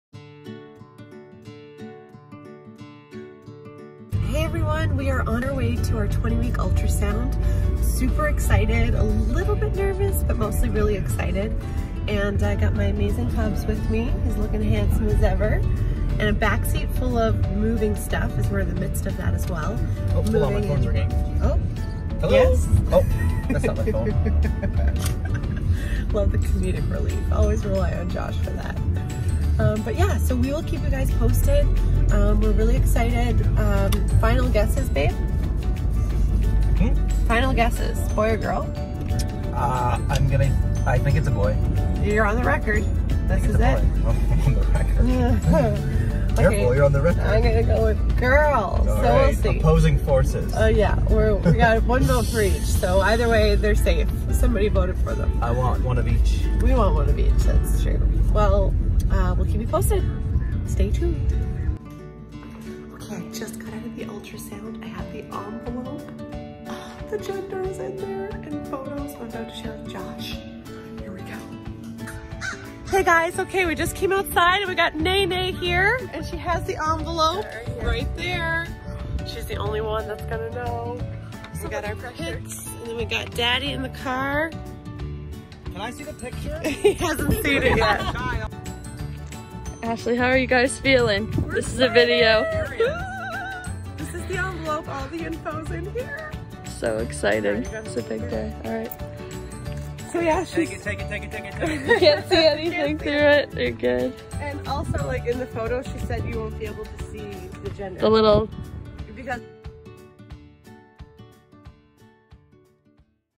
20 WEEK ULTRASOUND!
Today was the day! We put together a few clips to keep you in the loop! The Gender is.